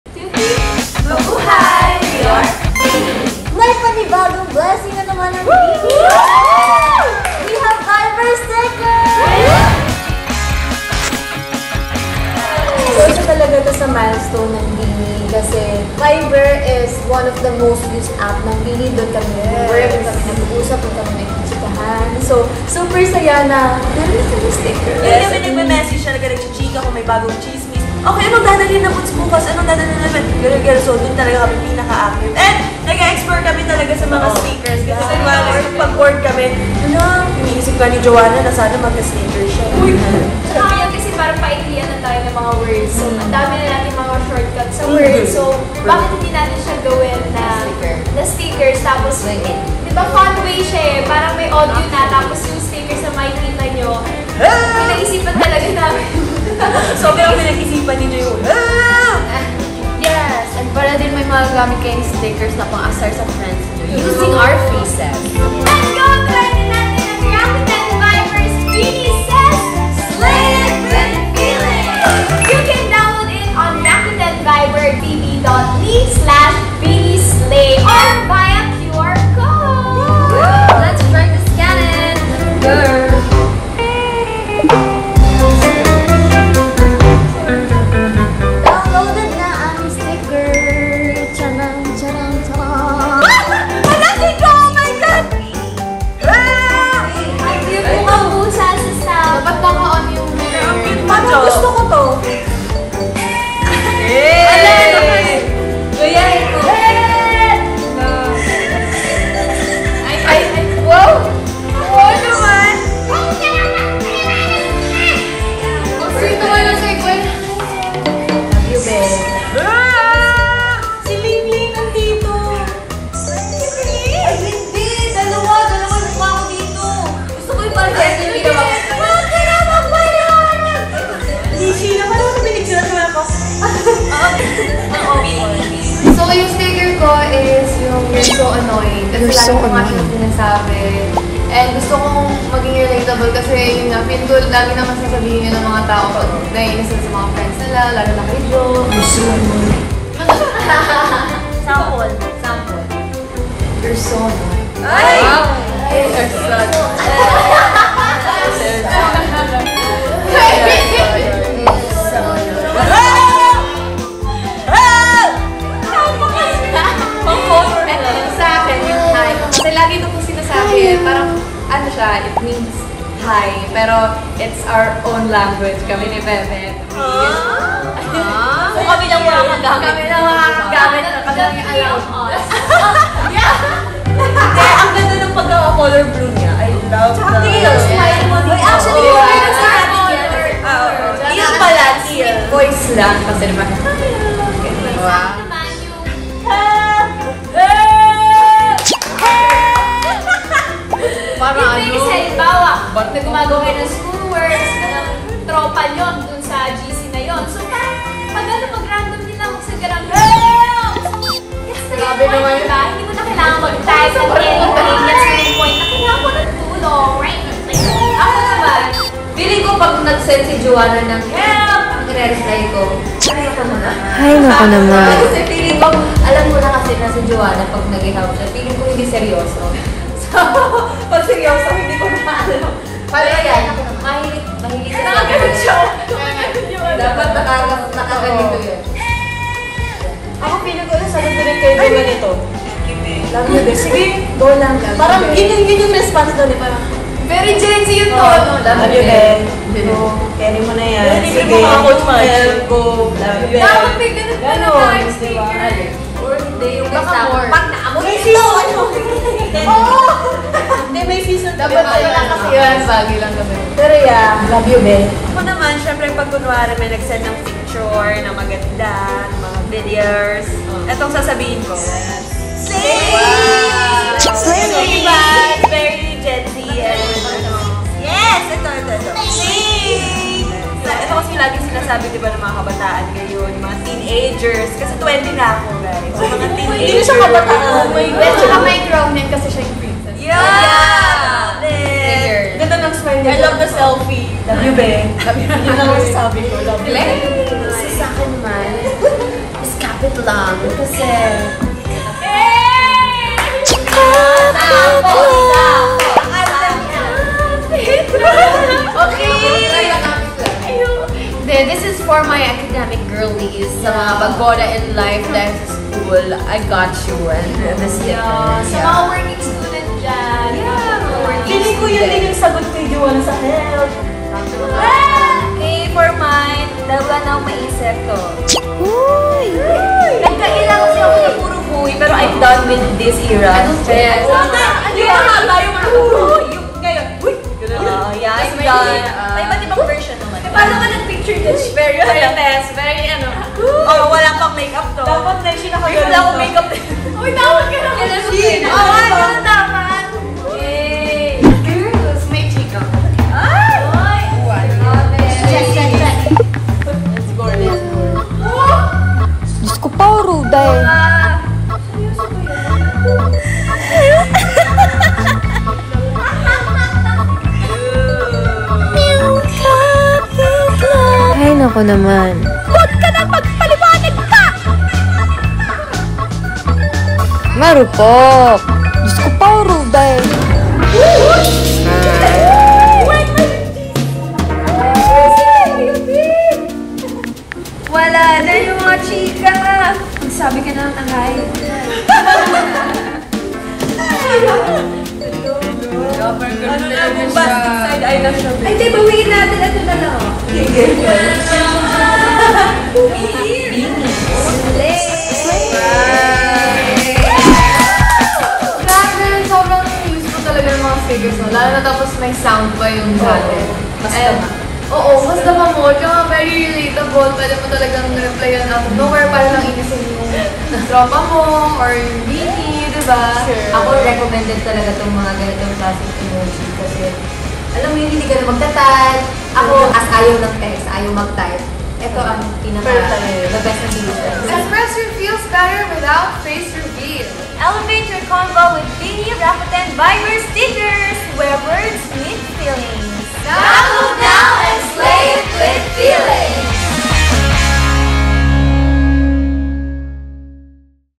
BINI : BINI Reacts to Rakuten Viber “BINI says… Slay It With Feelings”
BINI : Birds of the same feather, slay together! Kaya tara sabay-sabay nating panoodin ang BINI as they react to Rakuten Viber’s “BINI says… Slay it with Feelings!”! 💜

Kumu: bini_ph